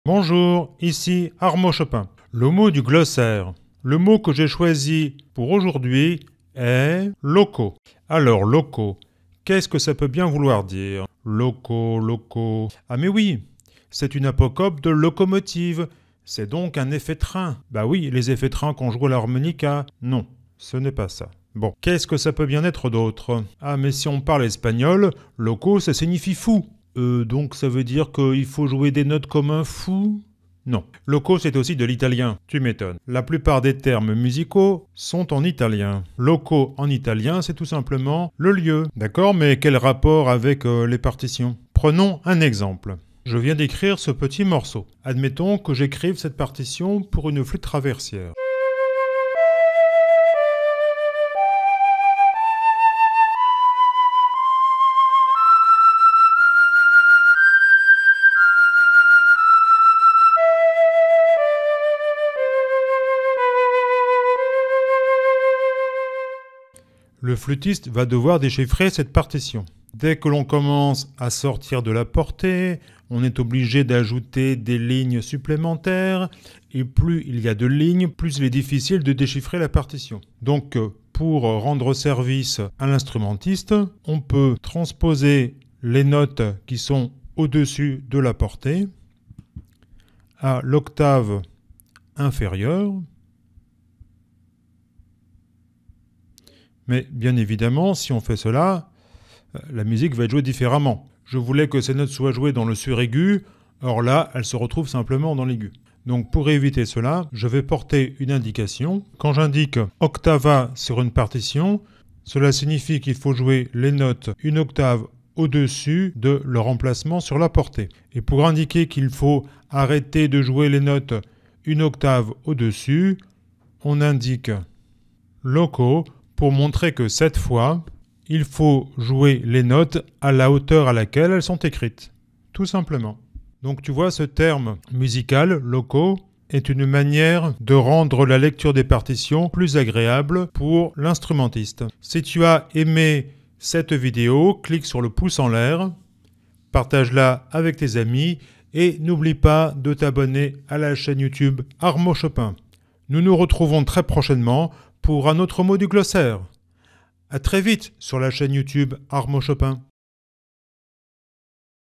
Solfège - Mot du Glossaire : LOCO
Aujourd'hui, je te montre ce que signifie le terme "loco" sur une partition.

Clique sur la cloche pour ne rien rater !